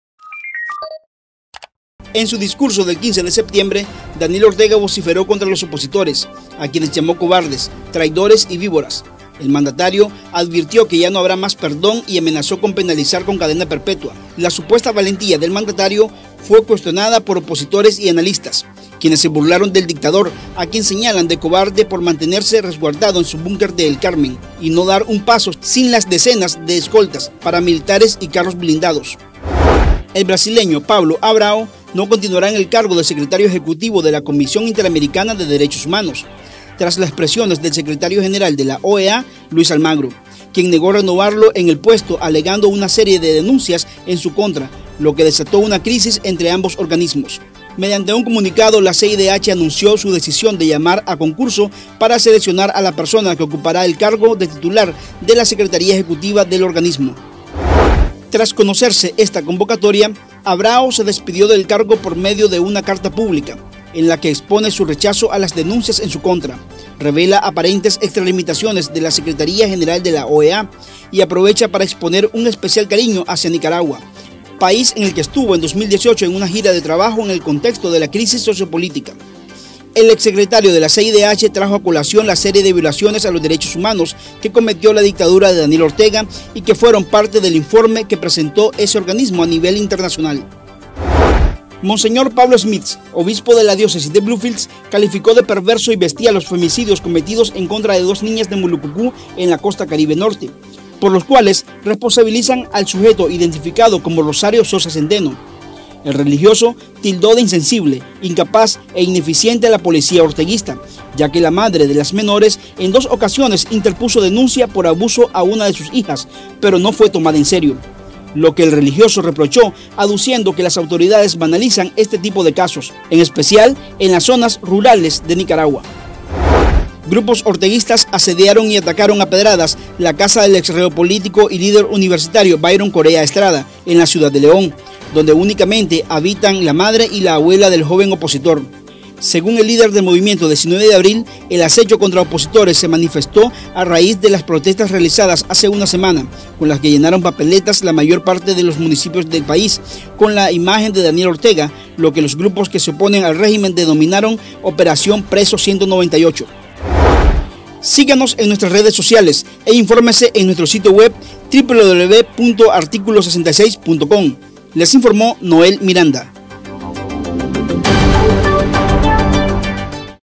Ortega llama «cobardes» a opositores en medio de decenas de escoltas y carros blindados
Opositores  y analistas se burlaron del dictador y lo señalaron de cobarde por mantenerse resguardado en su búnker de El Carmen y no dar un paso sin la decenas de escoltas, paramilitares y carros blindados